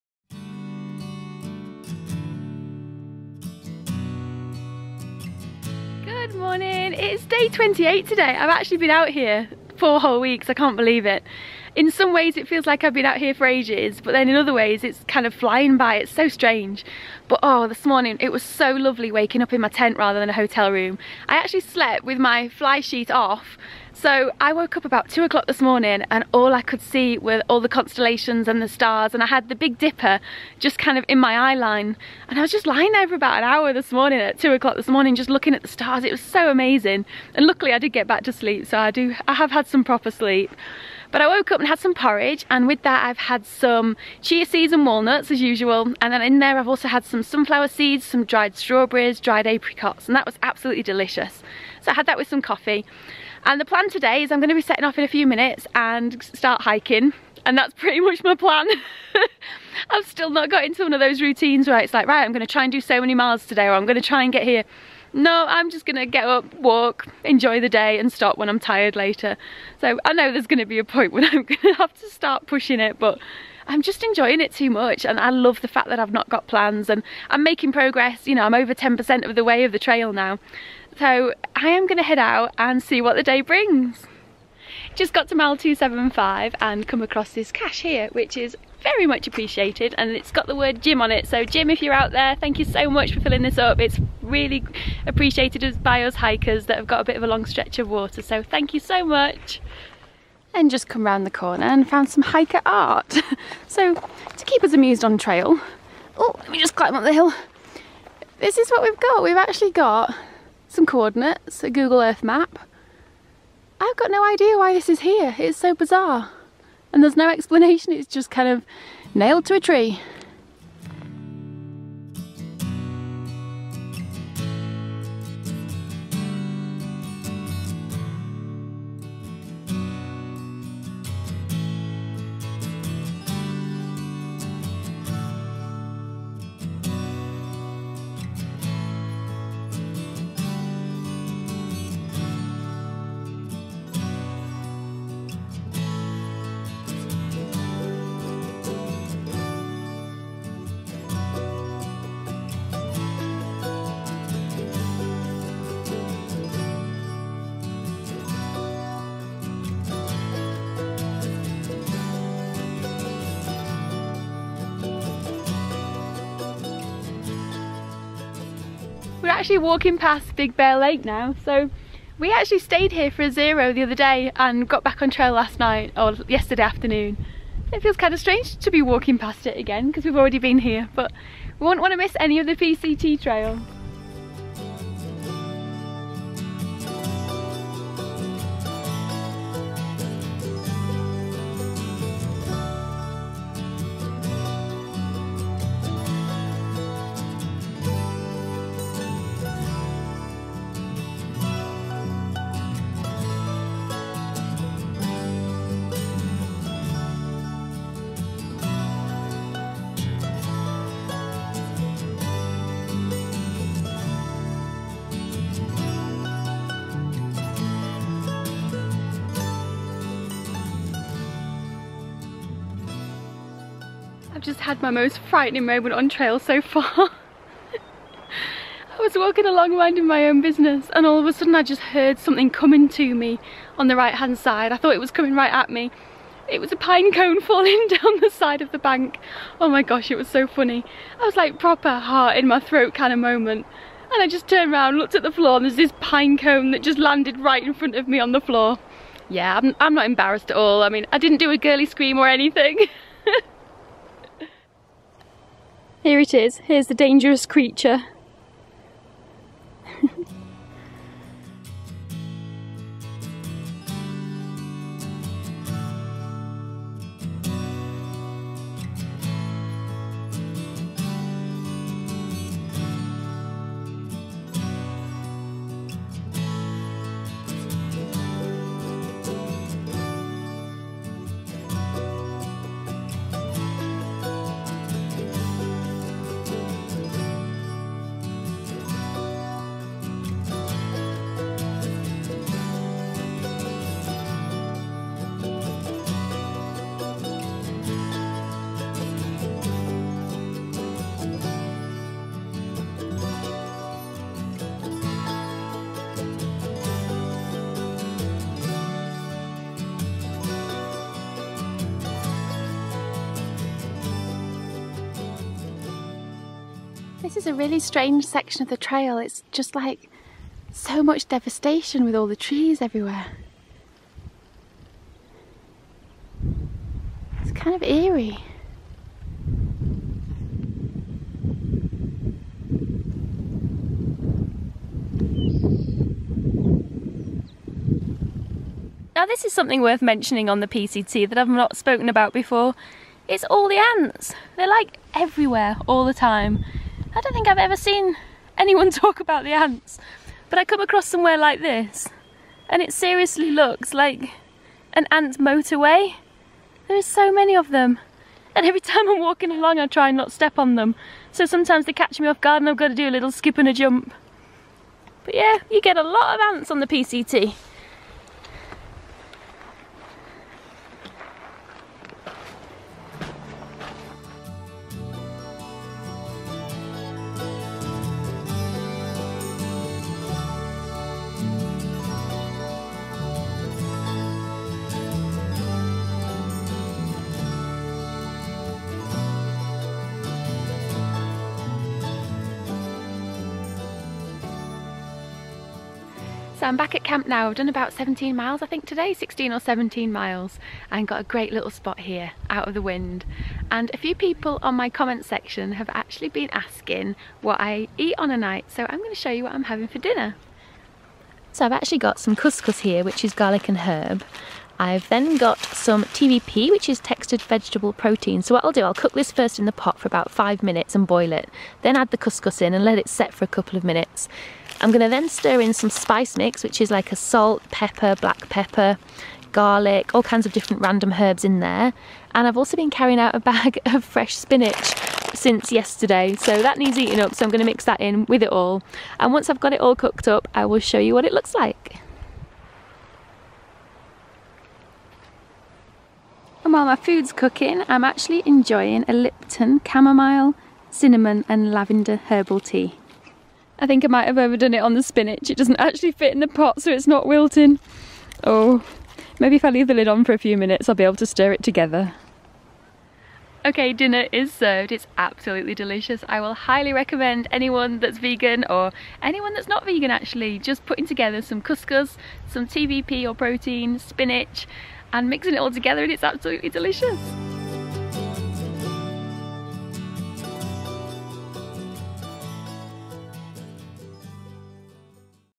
TRAUMATISED BY A PINE CONE & ANT MOTORWAY - DAY 28 | PCT 2019
Day 28 of my Pacific Crest Trail journey.

Filmed using;
↠ Canon PowerShot G7X MarkII
↠ Pedco Ultralight II Tripod

Thank you for taking the time to watch my video, I appreciate it so much.

Mary ❤